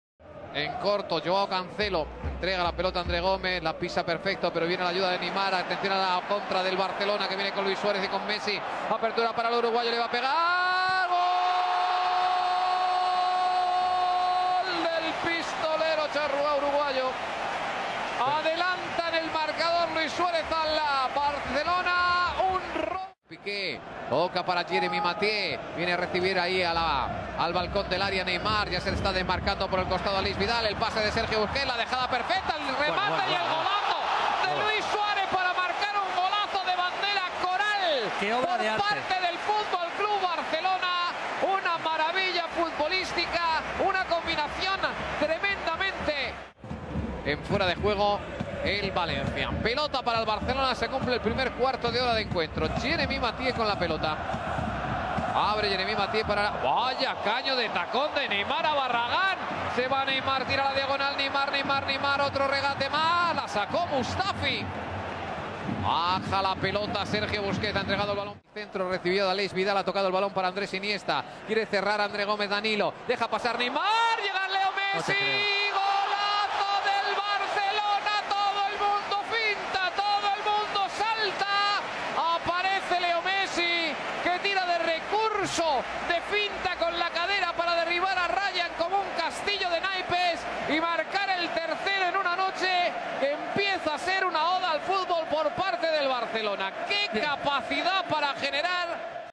FC Barcelona 7 vs 0 Valencia
Semi final CDR Leg 1 ( 4/2/2016 )
GOL : Suarez 4 & Messi 3
Assist : Neymar (2), Suarez (1), Vidal (1), Turan (1), Adriano (1)

Hasil pertandingan yang luar
 Biasa, para Sniper Barcelona berhasil menembak jatuh para Kelelawar Valencia 7x